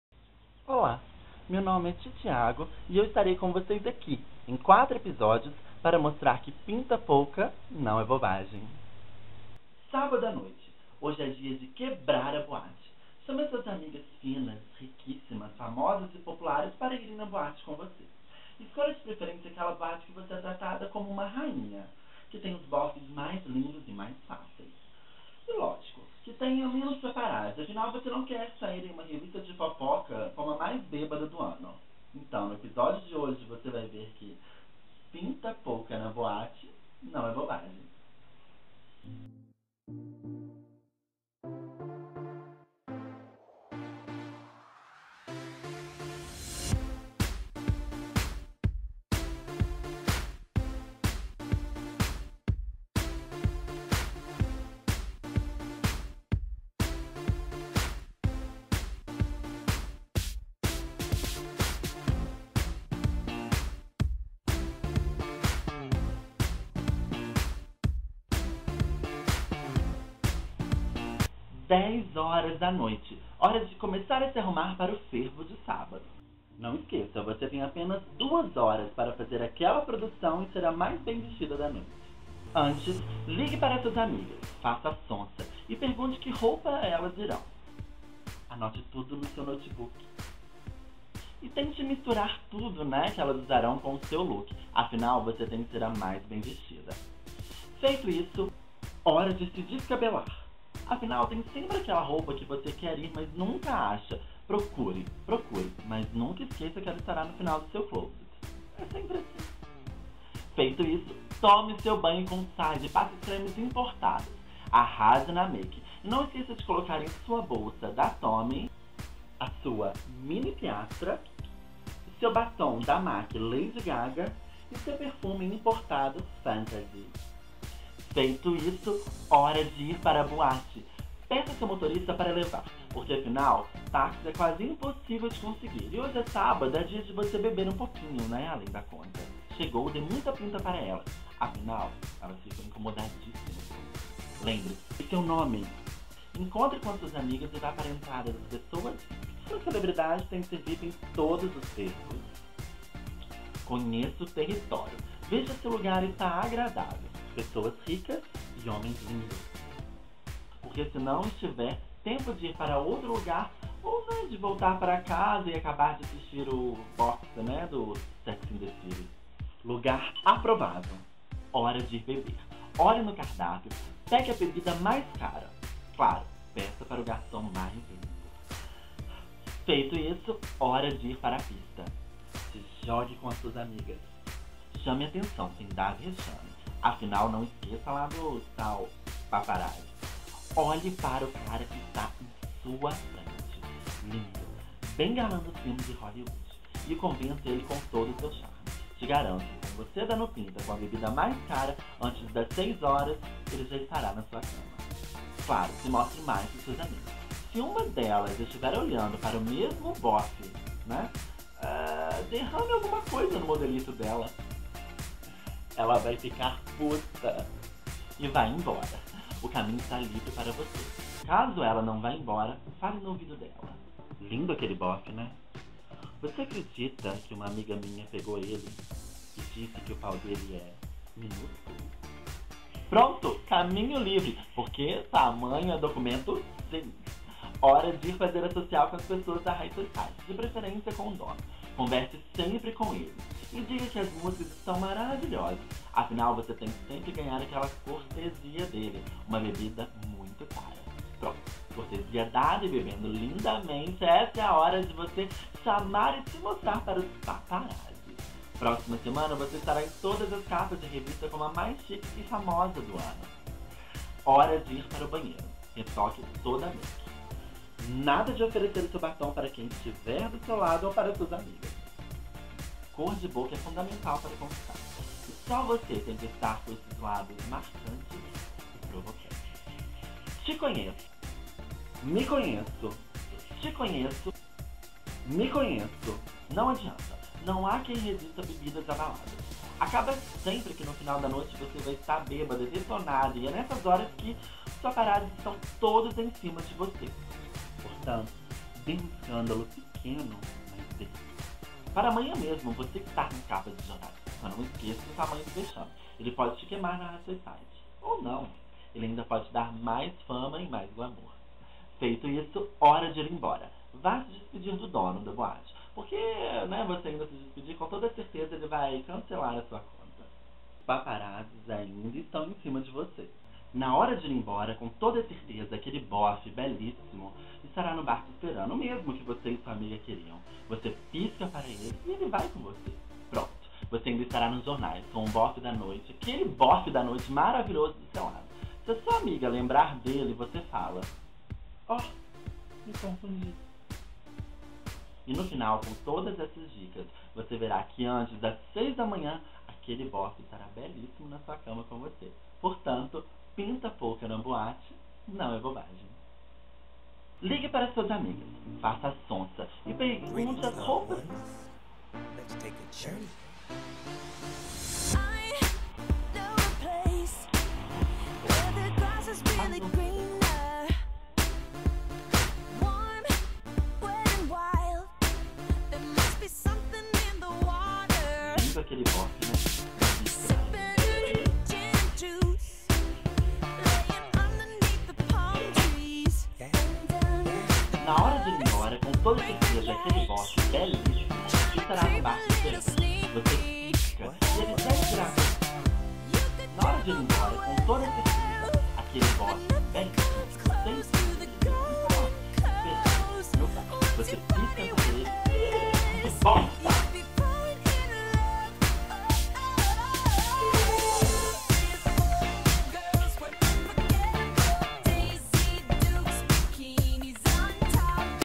"Pinta Pouca na BOATE Não é bobagem" Episódio 1 de 4
TITIago JF em seu Humor Chique, da dicas e revela que "Pinta Pouca NÃO é Bobagem!"
Confiram!
4 EPISÓDIOS!
(Texto Fictício, não real)